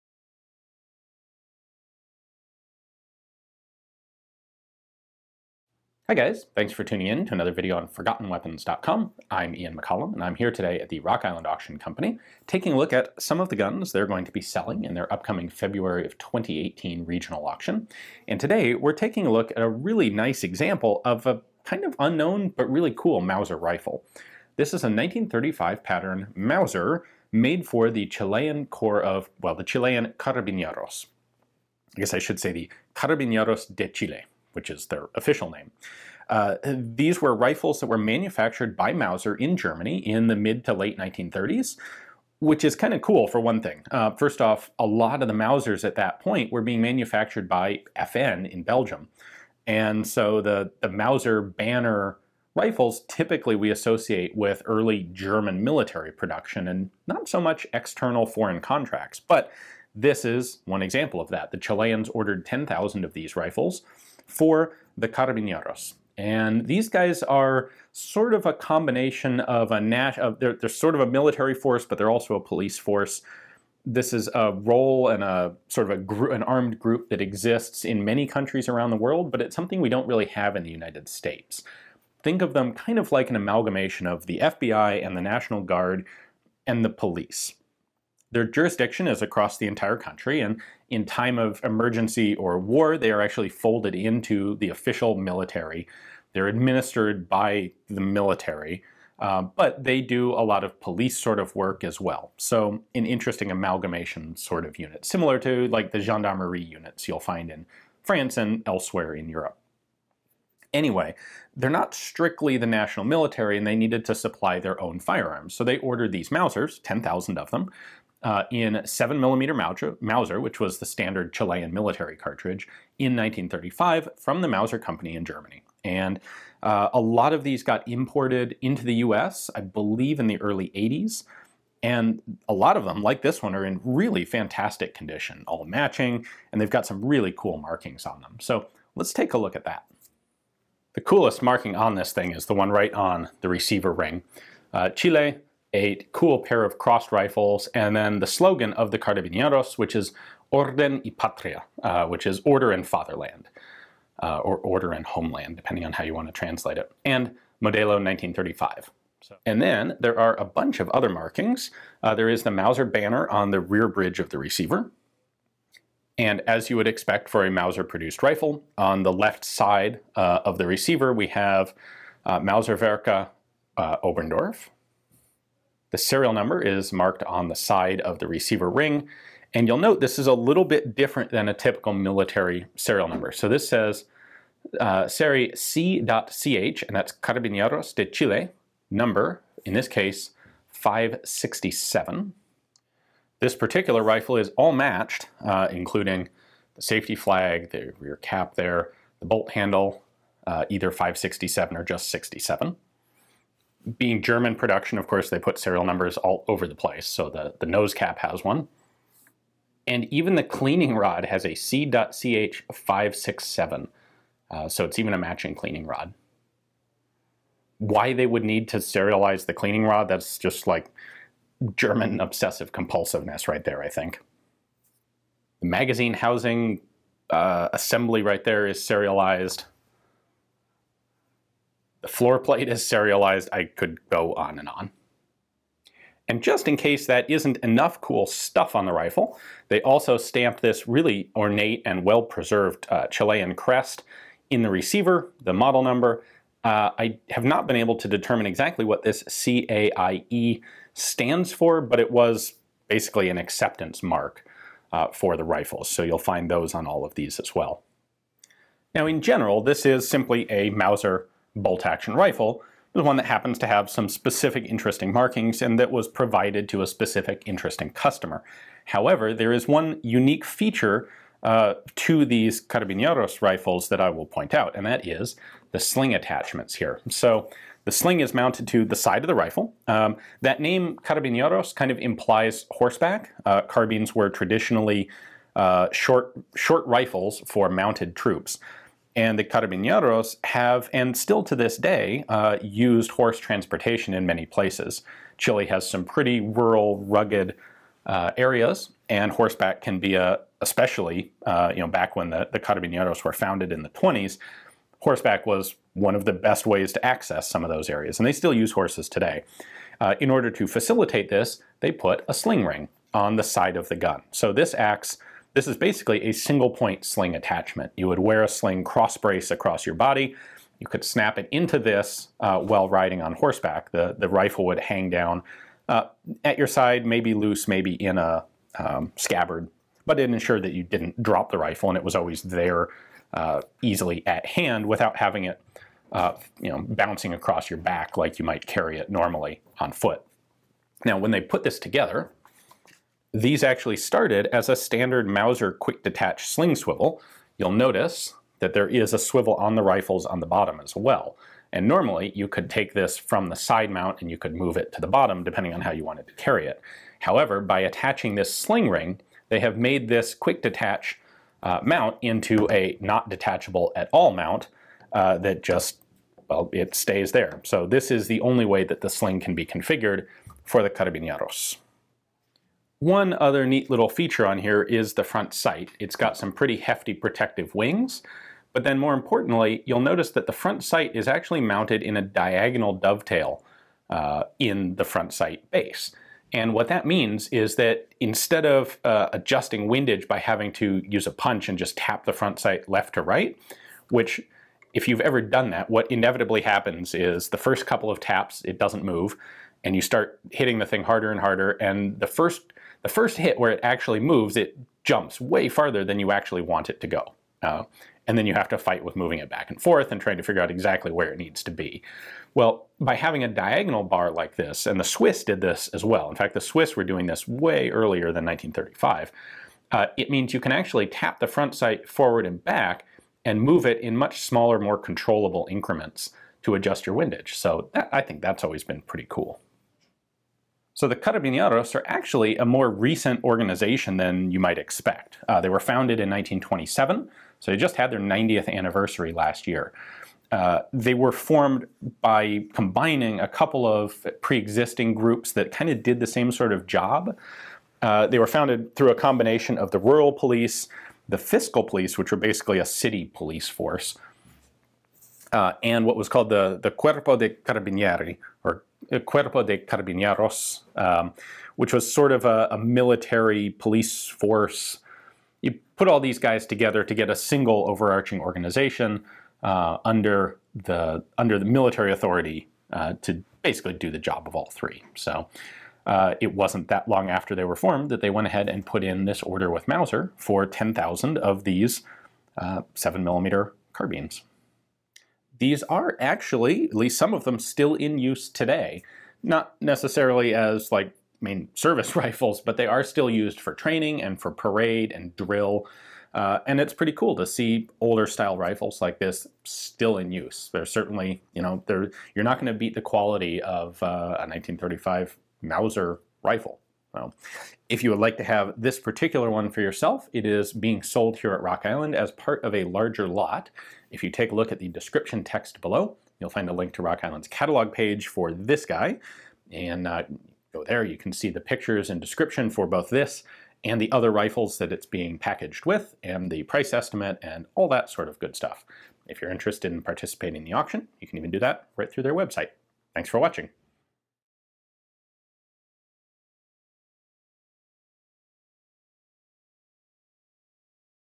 Orden Y Patria: Carabineros de Chile Model 1935 Mauser
The Carabinieros de Chile were formed in 1927 by combining the Rural Police, Fiscal Police, and Corps of Carabinieros into a single national police organization. We do not have an organization like this in the United States, but they are fairly common elsewhere in the world, acting as sort of a combination of National Guard, FBI, and police.

At any rate, the Carabinieros needed rifles, and in 1935 they placed an order for 10,000 carbines from Mauser in Oberndorf. These were chambered for 7x57mm Mauser (the standard Chilean military cartridge) and were all delivered prior to the outbreak of World War Two. They are excellent quality rifles, and feature a number of unique markings, most notable the receiver crest of crossed rifles over the Carbinieros motto, “Orden Y Patria”. They also have a unique sling arrangement, with the original Mauser quick-detach fitting fixed permanently in place by the addition of a saddle ring for mounted use.

This particular example is all matching, including the cleaning rod, and has very nice distinct stock markings.

Forgotten Weapons
6281 N Oracle #36270